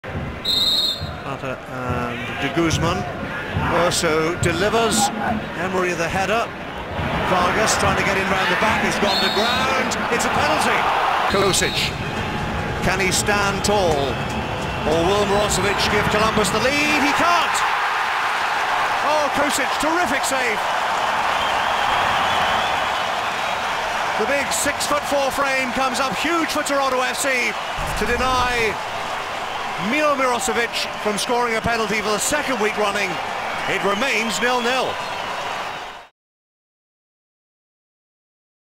Penalty Kick Save - Kocic blanks Columbus
Toronto FC Keeper Milos Kocic stretches low and wide to deny Columbus Crew midfielder Milovan Mirosevic.